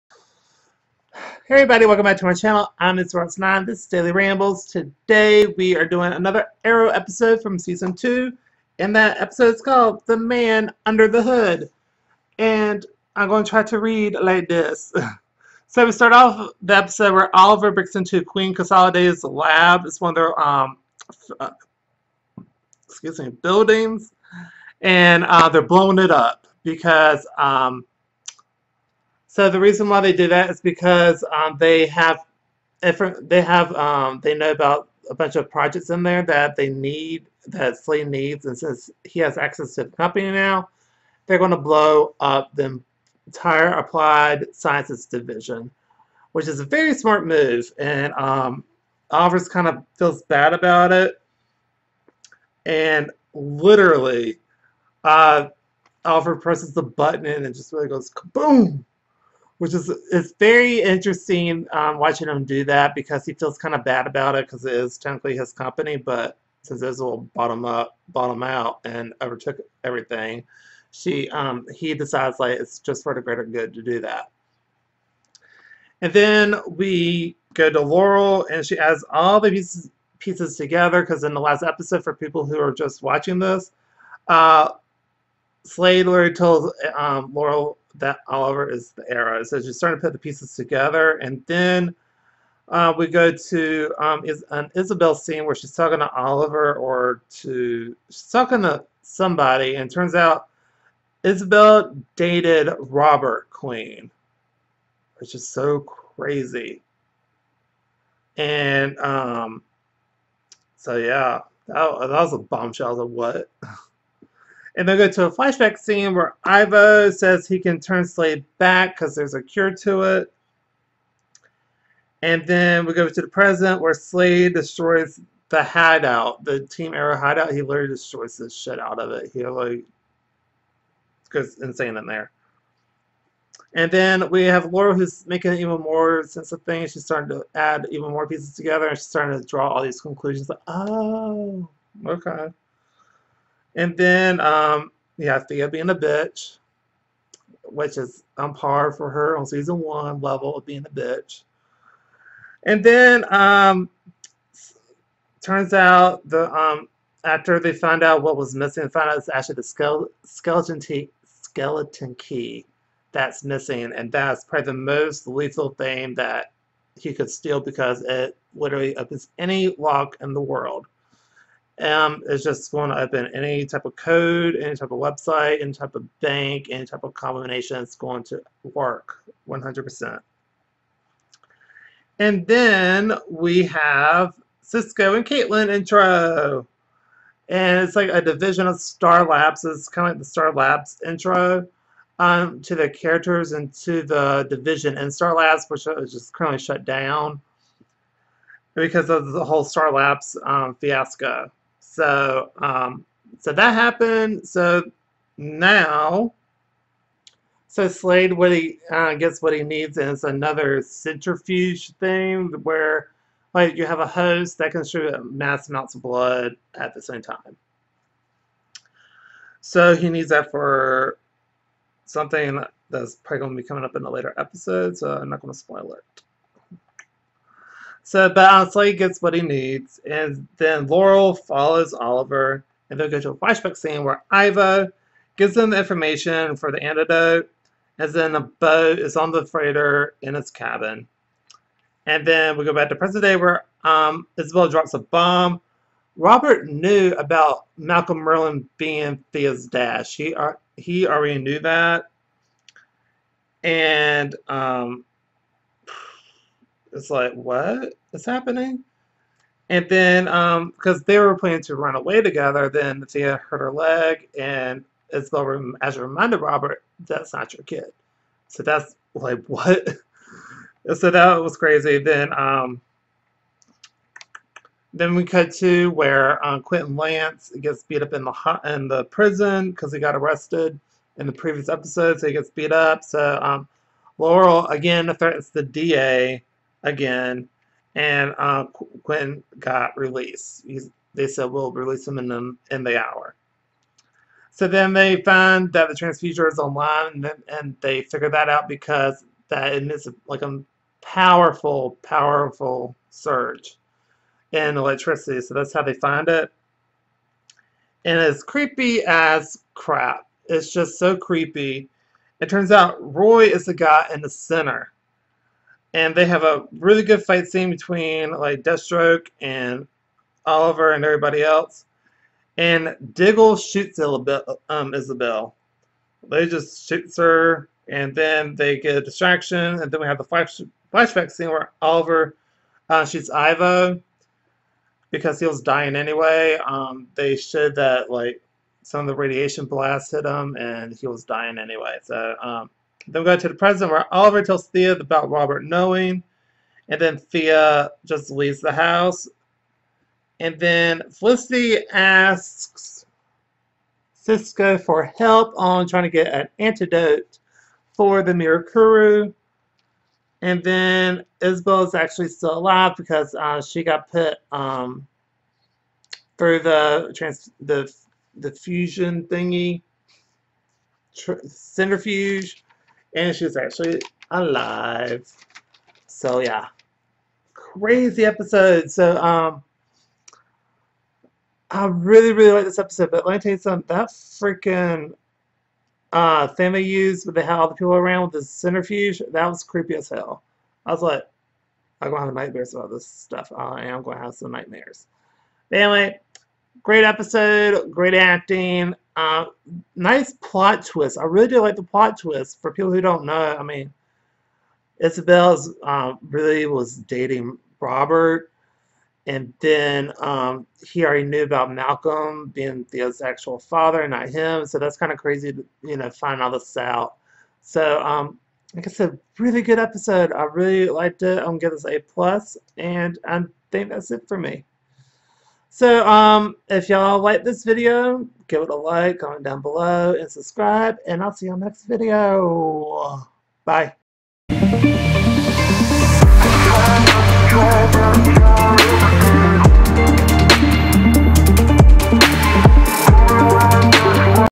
Arrow Season 2 Episode 19 The Man Under The Hood REVIEW
Let's turn out some videos shall we? I hope you all enjoy and just remember I am super new at this and my setup isn't the best but these are so fun to do. Also, leave some suggestions in the comment section on what I should react to next.